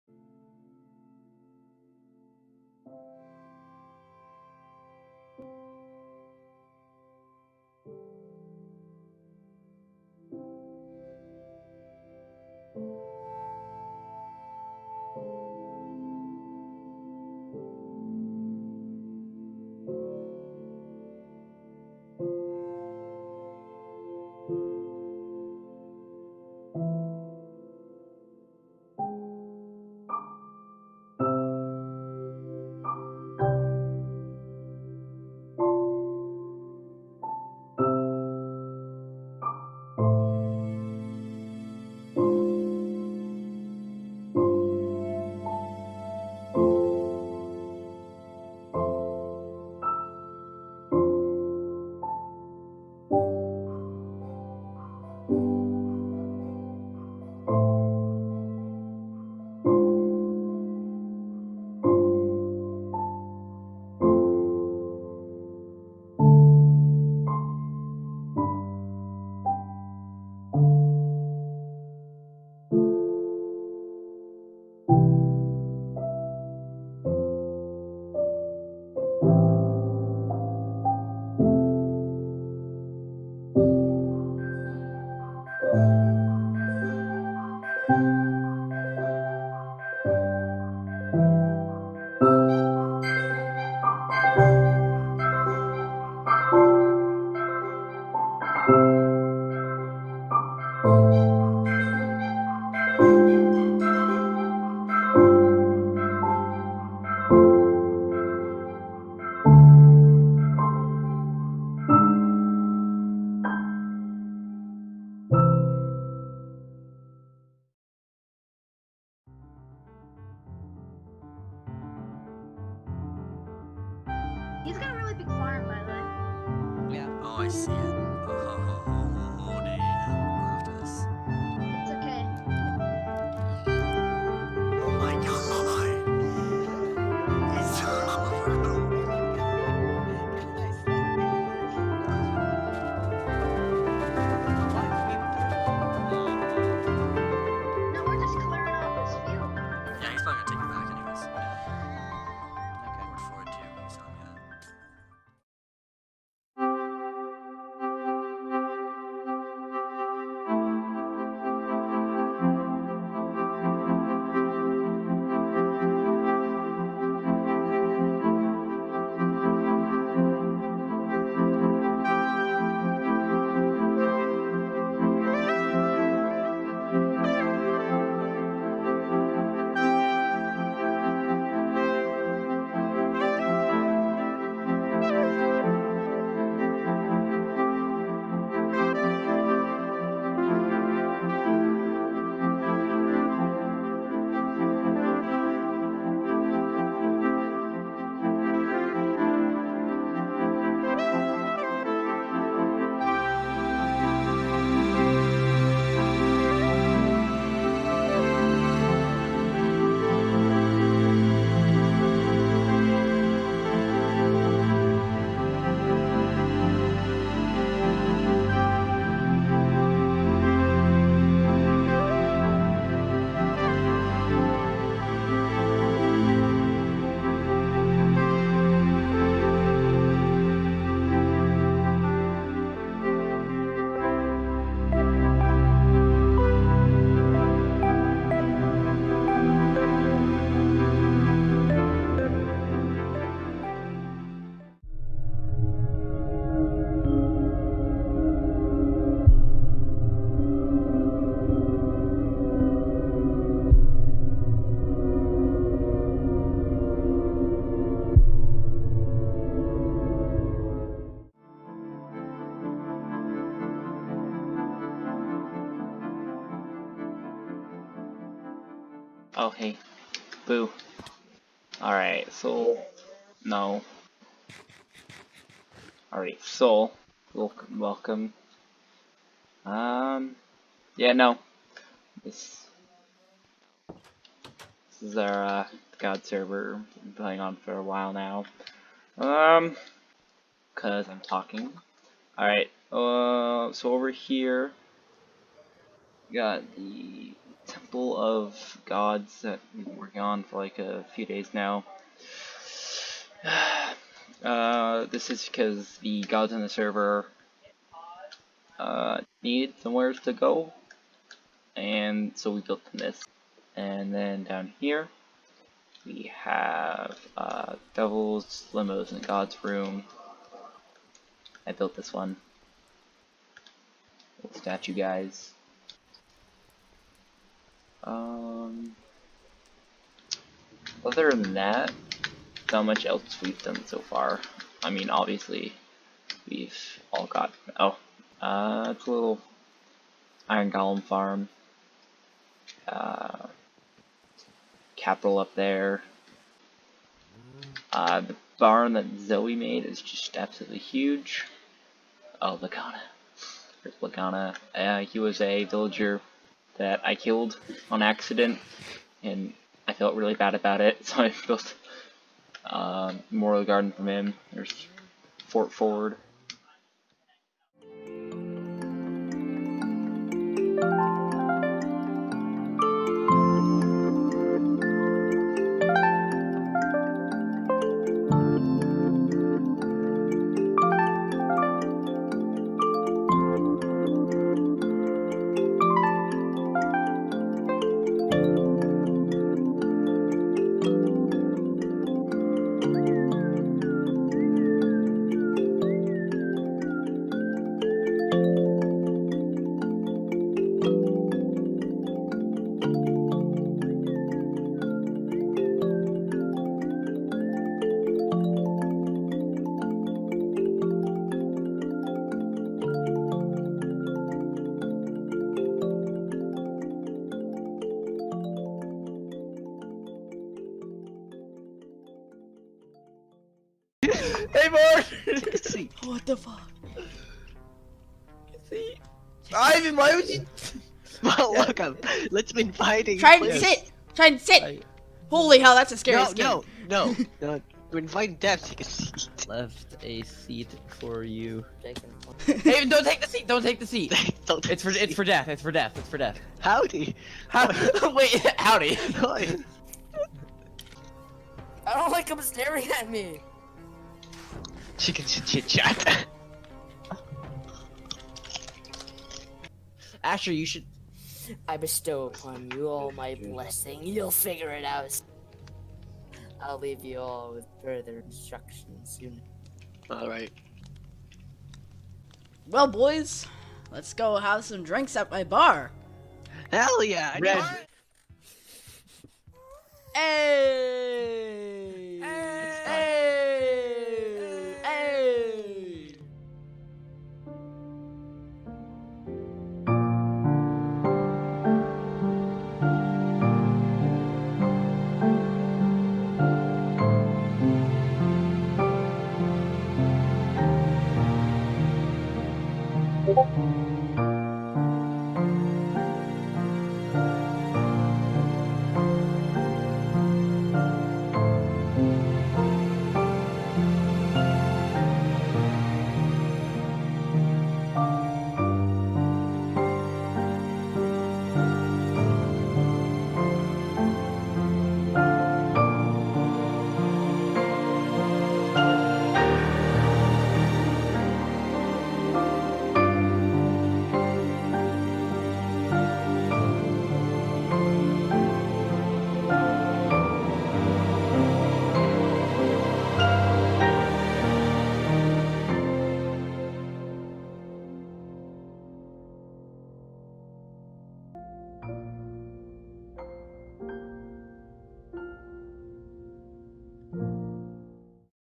Realms of the Gods: Journey Down Memory Lane
(The change in screenshots is loosely based on the music)

I devote this video to my friends who've made these years fun, and especially from the source of my favorite memories, which is Realms of the Gods. As I've gotten older I've realized how important it is to cherish these older memories and that's why this video exists and why I even upload videos. I don't know if we'll do anything like this in the future but at least I can say we had an amazing run of memories. I've noticed how quickly we as a group have burned out our imaginative narratives and ambitions towards certain things, and this being one of them. if we do ever see anything like this series again it will be one for the books, that's for sure.

Sorry I didn't have much actual footage of our earlier years, I'm basically stuck with screenshots we've kept over the years. By the way, If I've messed up any of the photo captions or the year of said screenshot, let me know.